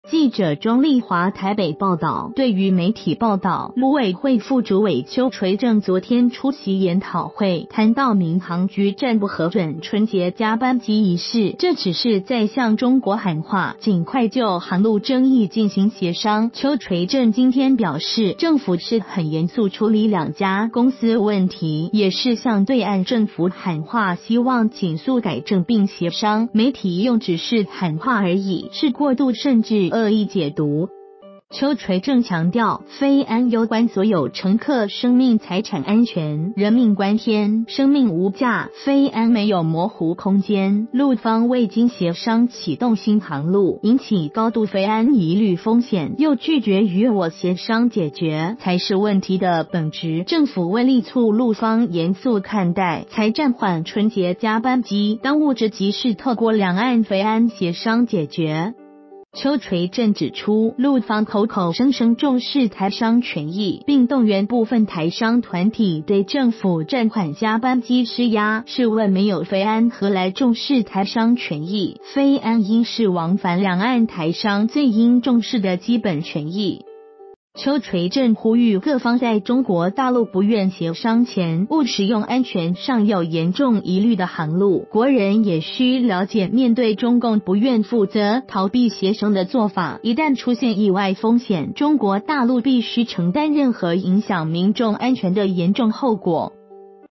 暫緩春節加班機只是喊話？ 邱垂正籲兩岸儘速協商
〔記者鍾麗華／台北報導〕對於媒體報導，陸委會副主委邱垂正昨天出席研討會，談到民航局暫不核准春節加班機一事，這「只是」在向中國喊話，儘快就航路爭議進行協商。邱垂正今天表示，政府是很嚴肅處理兩家公司問題，也是向對岸政府喊話希望儘速改正並協商，媒體用「只是」喊話而已，是過度甚至惡意解讀。

發表評論評論如下。